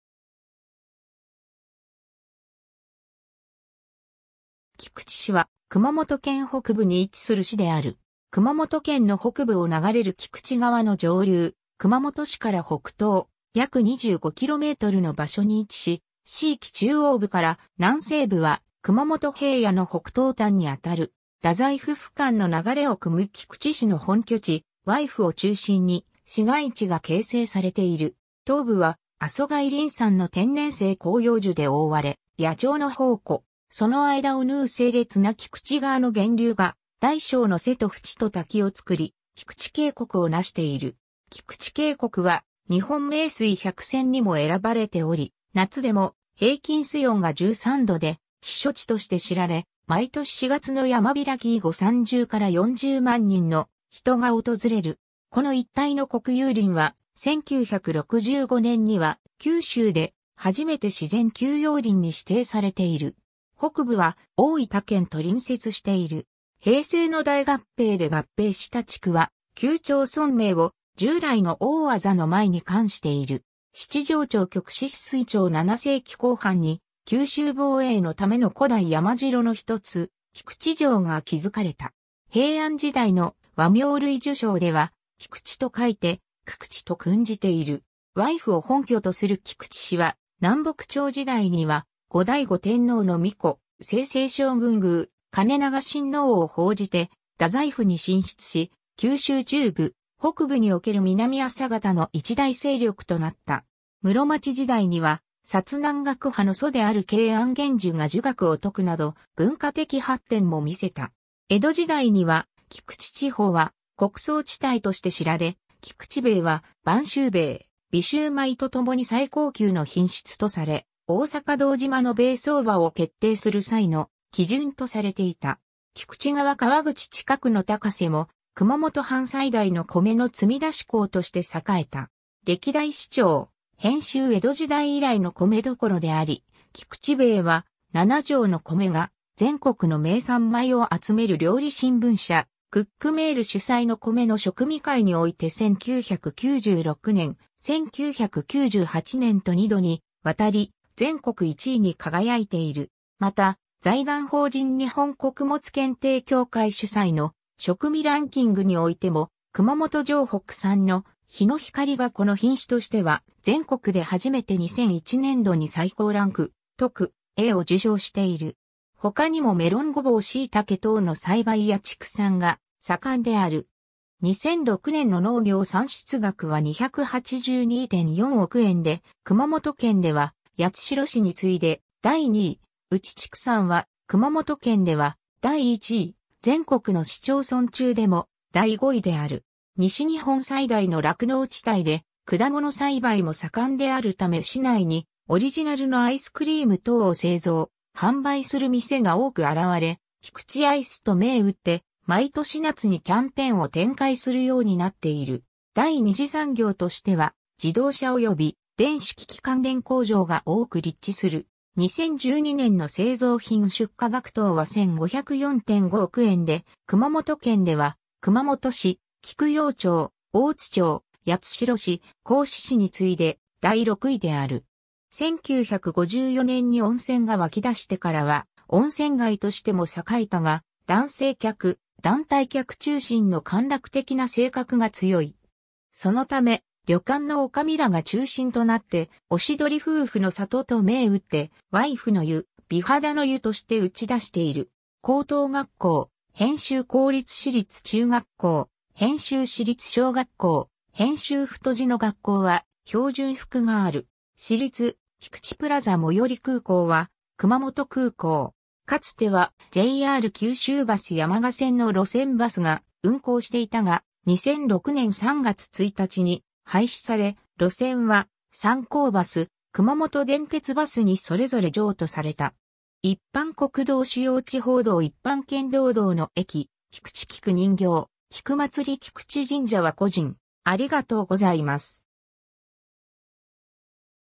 菊池市
菊池市（きくちし）は、熊本県北部に位置する市である。
 熊本県の北部を流れる菊池川の上流、熊本市から北東約25kmの場所に位置し、市域中央部から南西部は熊本平野の北東端にあたる。
大宰府府官の流れをくむ菊池氏の本拠地・隈府（わいふ）を中心に市街地が形成されている。
 東部は阿蘇外輪山の天然生広葉樹で覆われ、野鳥の宝庫。
その間を縫う清冽な菊池川の源流が大小の瀬と渕と滝をつくり菊池渓谷をなしている。
菊池渓谷は日本名水百選にも選ばれており、夏でも平均水温が13度で避暑地として知られ、毎年4月の山開き以後30 - 40万人の人が訪れる。
この一帯の国有林は、1965年には九州で初めて自然休養林に指定されている。
 北部は大分県と隣接している。
 平成の大合併で合併した地区は旧町村名を従来の大字の前に冠している。
 七城町 旭志(旧旭志村) 泗水町 7世紀後半に、九州防衛のための古代山城の一つ、鞠智城（きくちじょう）が築かれた。
平安時代の『倭名類聚抄』では「菊池」と書いて「久々知」（くくち）と訓じている。
 隈府を本拠とする菊池氏は、南北朝時代には後醍醐天皇の皇子・征西将軍宮懐良親王を奉じて大宰府に進出し、九州中部・北部における南朝方の一大勢力となった。
室町時代には薩南学派の祖である桂庵玄樹が儒学を説くなど、文化的発展も見せた。
 江戸時代には、菊池地方は穀倉地帯として知られ、菊池米（肥後米）は、播州米・備州米とともに最高級の品質とされ大坂堂島の米相場を決定する際の基準とされていた。
菊池川河口近くの高瀬（現・玉名市）も、熊本藩最大の米の積出港として栄えた。
 歴代市長[編集] 江戸時代以来の米どころであり、菊池米は、「七城のこめ」が全国の名産米を集める料理新聞社「クックメール」主催の米の食味会において1996年、1998年と2度に渡り全国1位に輝いている。
また、財団法人日本穀物検定協会主催の食味ランキングにおいても「熊本 城北」産のヒノヒカリがこの品種としては全国で初めて2001年度に（その後2005年度、2008年度～2015年度にも）最高ランク「特A」を受賞している。
 他にもメロン・ゴボウ・シイタケ等の栽培や畜産が盛んである。
2006年の農業産出額は282.4億円で、熊本県では八代市に次いで第2位、うち畜産は熊本県では第1位、全国の市町村中でも第5位である（なお2007年以降、農林水産省は市町村レベルの農業産出額統計を作成していない）。
 西日本最大の酪農地帯で果物栽培も盛んであるため市内にオリジナルのアイスクリーム等を製造・販売する店が多く現れ、菊池アイスと銘打って毎年夏にキャンペーンを展開するようになっている。
 第二次産業としては、自動車および電子機器関連工場が多く立地する。
2012年の製造品出荷額等は1504.5億円で、熊本県では熊本市、菊陽町、大津町、八代市、合志市に次いで第6位である。
 1954年に温泉が湧出してからは温泉街としても栄えたが、男性客、団体客中心の歓楽的な性格が強い。
そのため、旅館の女将らが中心となって「おしどり夫婦の里」と銘打って、「わいふ（隈府、ワイフ=妻）の湯」「美肌の湯」としてうち出している。
 高等学校[編集] 公立 私立 中学校[編集] 市立 小学校[編集] 太字の学校は標準服がある。
 市立 菊池プラザ 最寄り空港は熊本空港（阿蘇くまもと空港）。
 かつてはJR九州バス山鹿線の路線バスが運行していたが、2006年3月1日に廃止され、路線は産交バス・熊本電鉄バスにそれぞれ譲渡された。
 一般国道 主要地方道 一般県道 道の駅 菊池菊人形・菊祭り 菊池神社 ★は故人